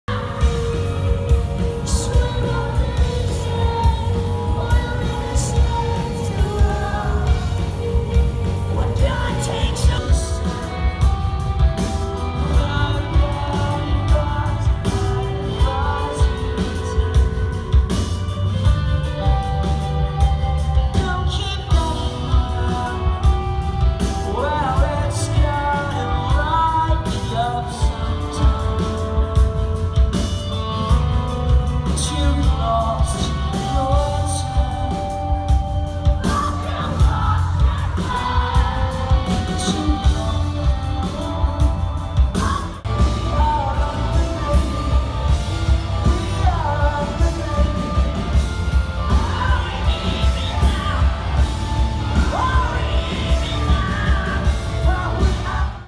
Circa Survive, Live from someone else's phone and camera!
+ Bonus vertical video footage of someone recording a vertical video! RAD 2XTREME!!!